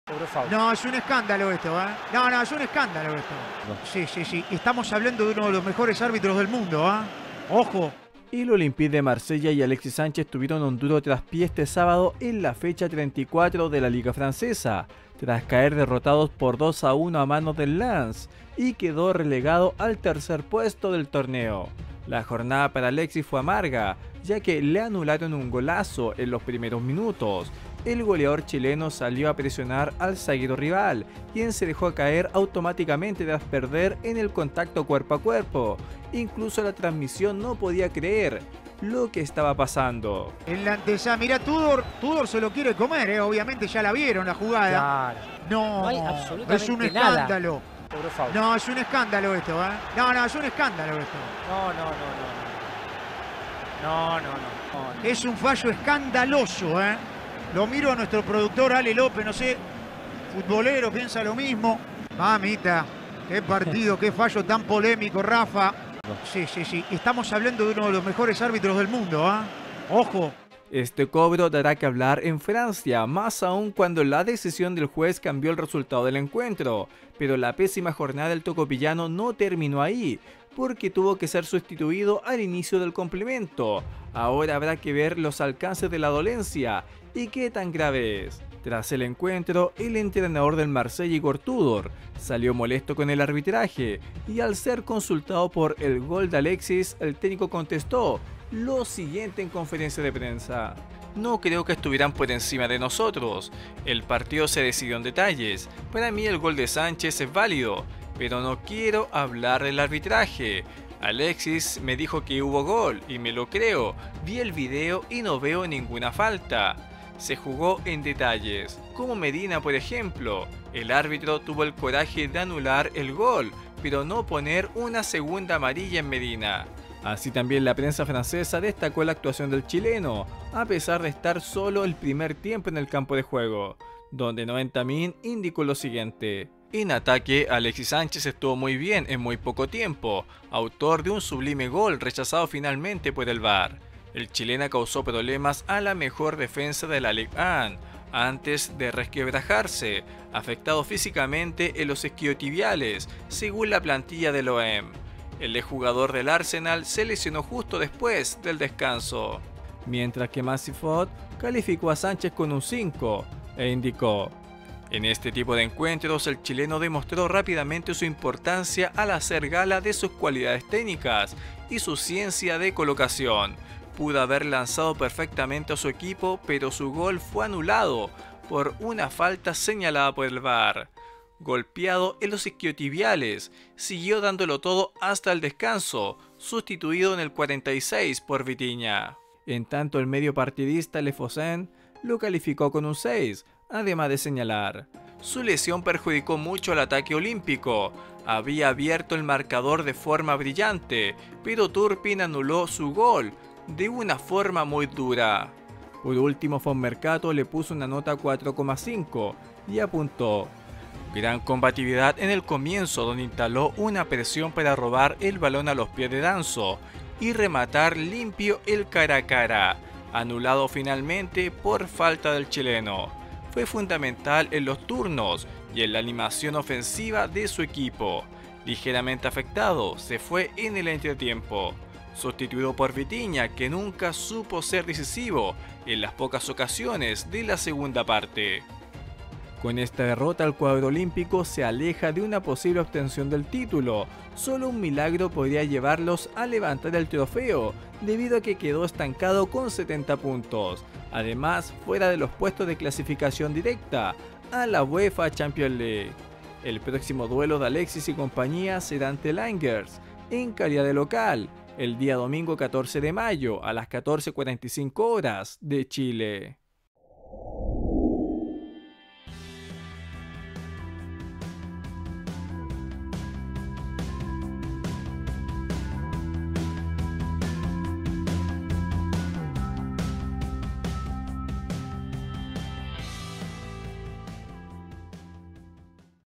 Prensa NO Puede Creer Gol Anulado de Alexis Sánchez en Derrota de Marsella Ante Lens
Prensa NO Puede Creer Gol Anulado de Alexis Sánchez en Derrota de Olympique de Marsella vs  Lens